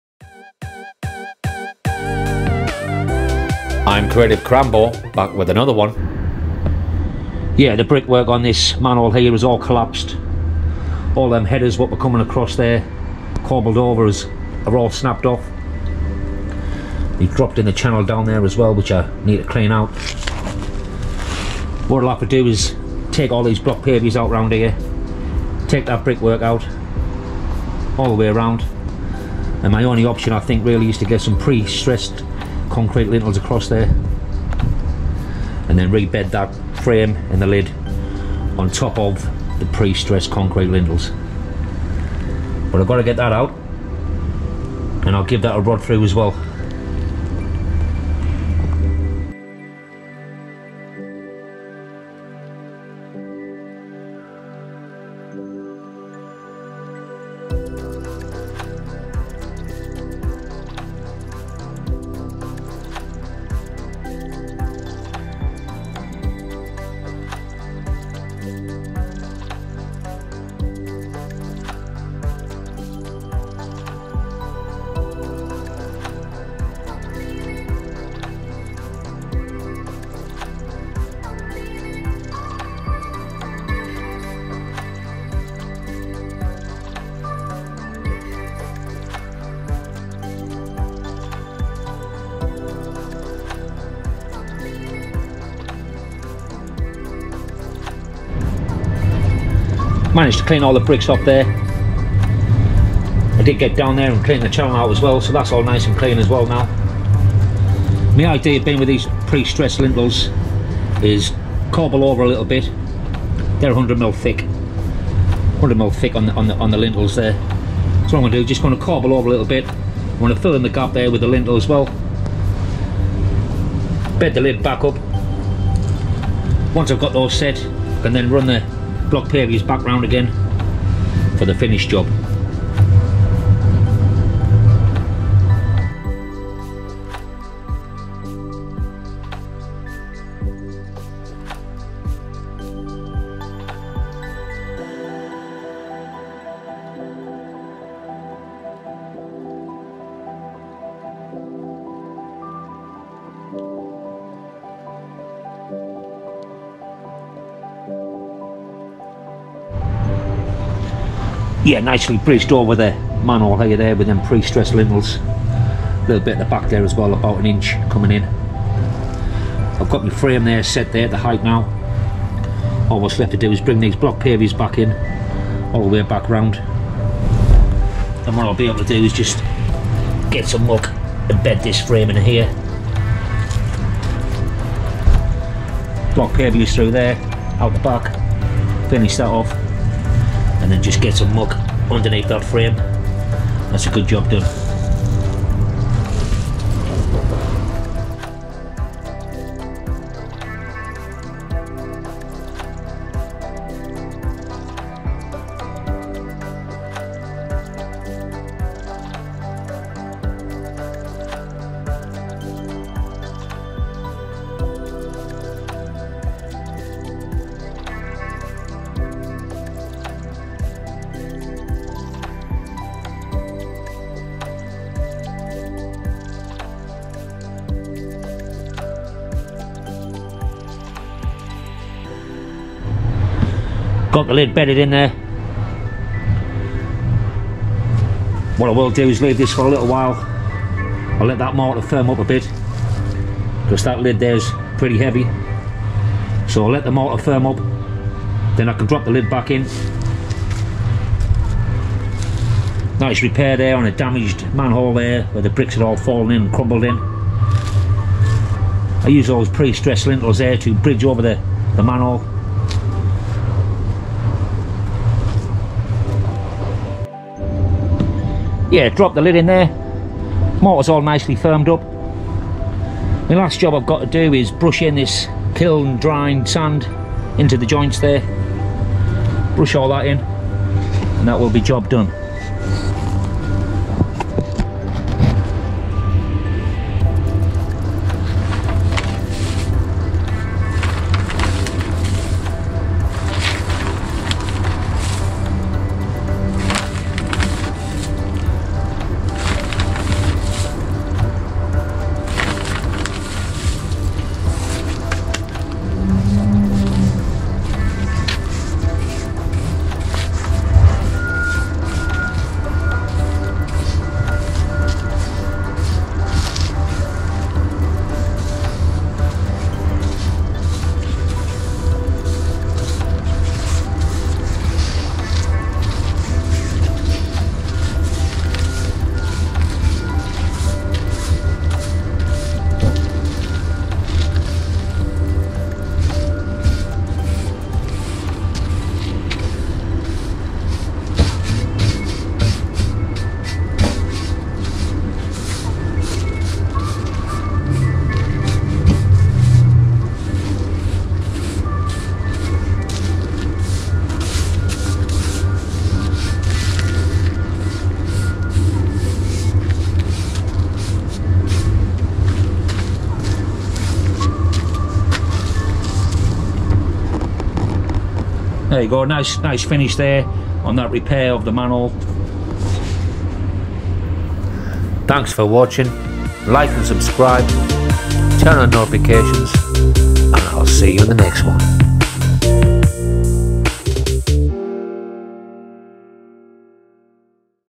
Manhole Repair Using Prestressed Concrete Lintels
In this video, I show you how I repair a collapsed manhole cover.

The brickwork that was supporting the frame had collapsed into the manhole and the chamber below, this was probably caused by heavy traffic going over the manhole cover so I had to strip it all back to allow for some prestressed lintels to go across the manhole and bridge the manhole,  so I could set the frame back in. I used prestressed lintels to do this. A prestressed lintel means they have heightened strength by the way of steel wires which have been set to high tension, running through the middle of the lintel.
They are designed to support structural loadings above openings in internal and external masonry walls and worked perfectly for bridging over the manhole so I could set the frame back in and do the repairs.